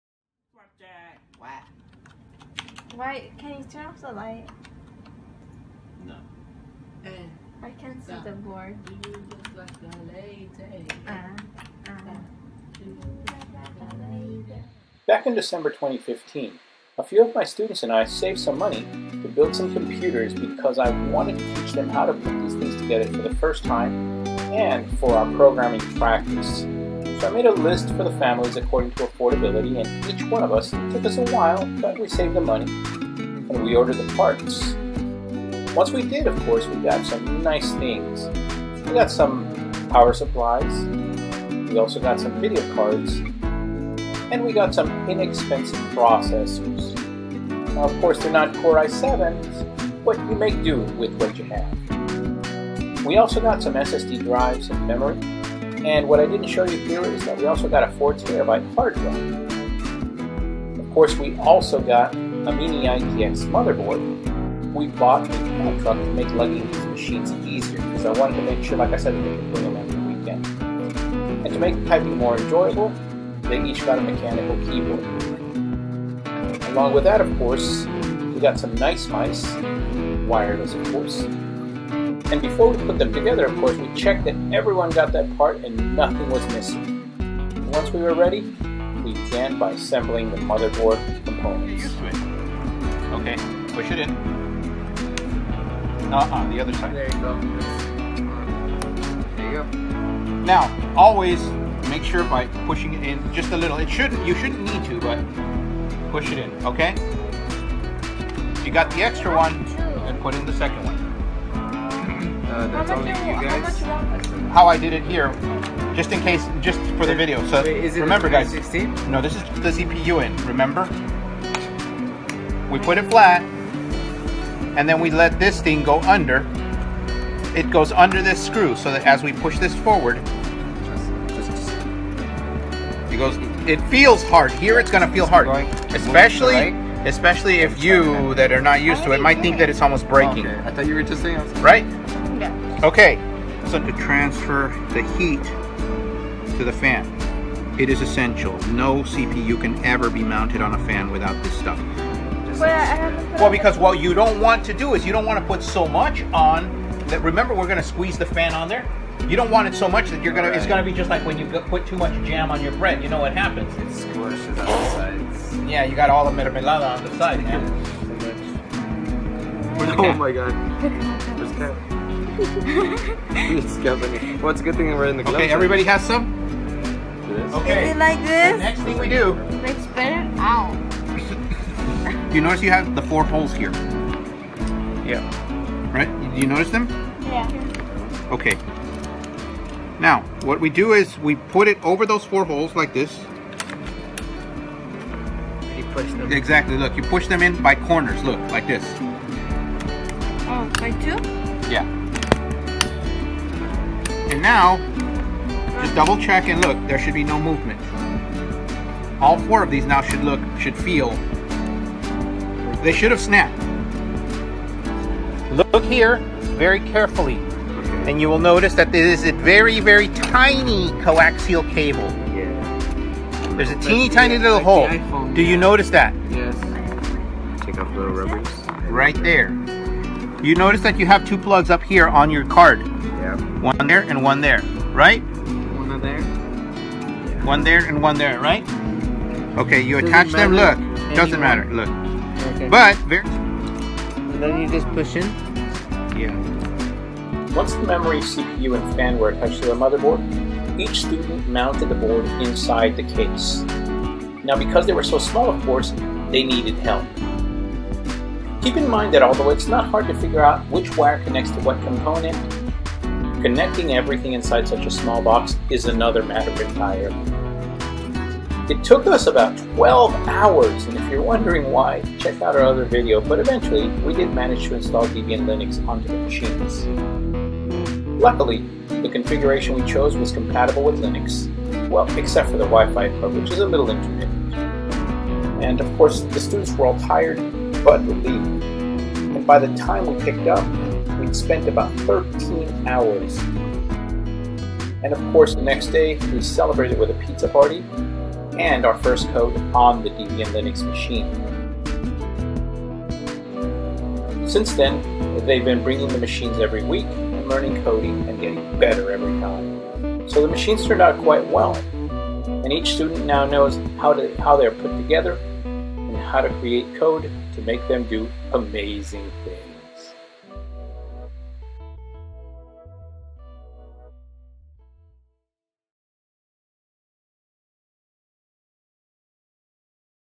Teaching kids to build a mini-itx pc
In December 2015, a bunch of my students saved enough money (it took months) to build their own PCs. We needed a PC that could be transported, yet powerful (so no laptops) and expandable. We settled on a mini-ITX machine with a Pentium G3258, an SSD, an HDD, a modest video card, a mechanical keyboard, and we installed Debian Linux on the machine. It took us about 12 hours to build the machines (you try getting a bunch of kids to do it) but in the end, they built the machines with their own hands, and now use them for coding practice.